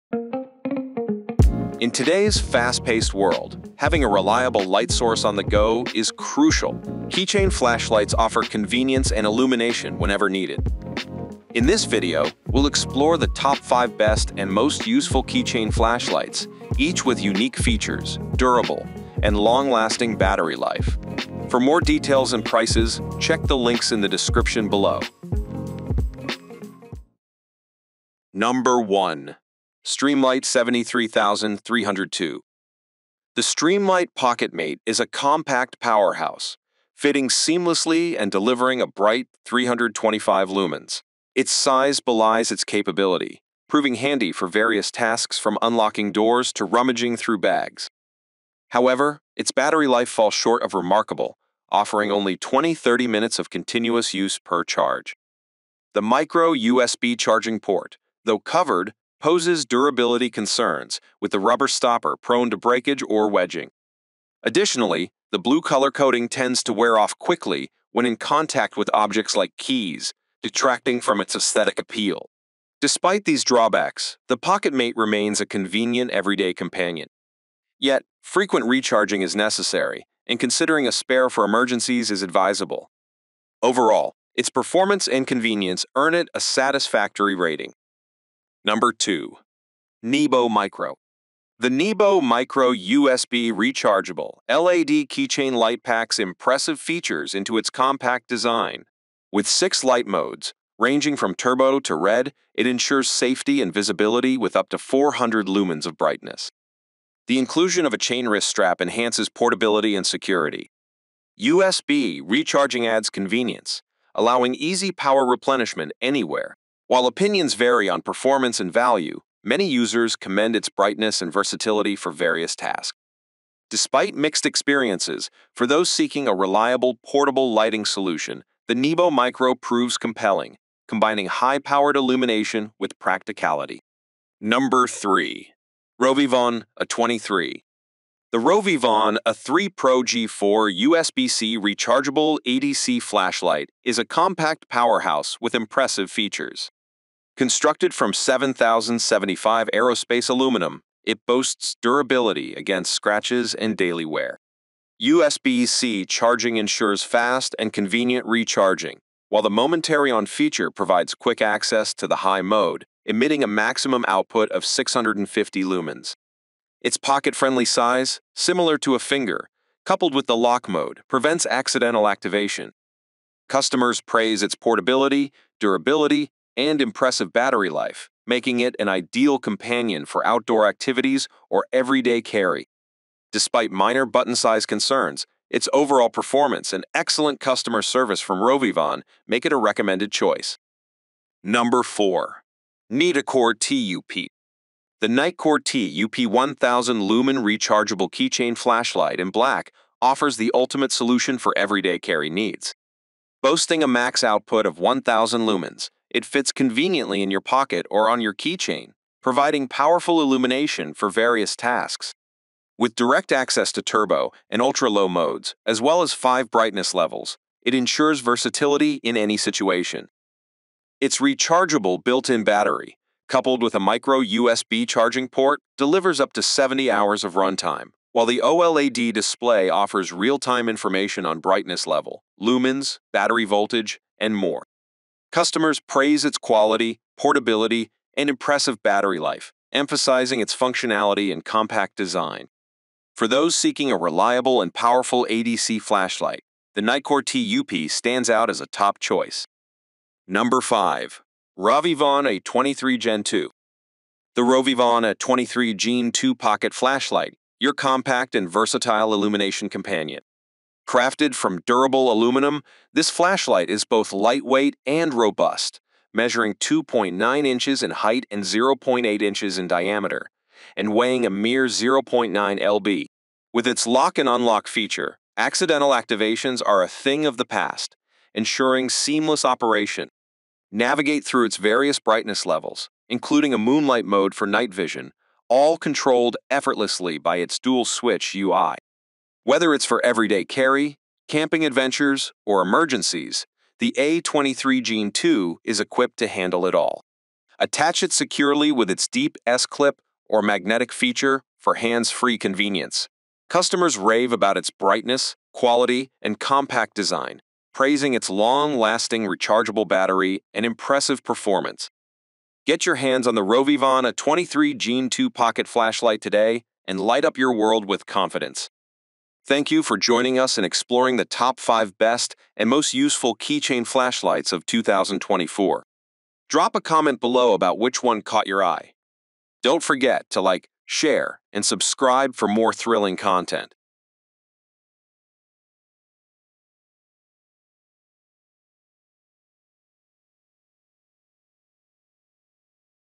Best Useful Keychain Flashlight 2024 (Top 5 Picks) - Best EDC Flashlight
Looking for the best useful keychain flashlight in 2024? Check out our top 5 picks for the best EDC flashlight, featuring brands like Nitecore and Rovyvon. Ideal for everyday carry and emergencies!

✅ Purchase Links:
1. Streamlight 73302

2. Nebo Mycro

3. RovyVon A23

4. Nitecore TUP

5. RovyVon A23 gen2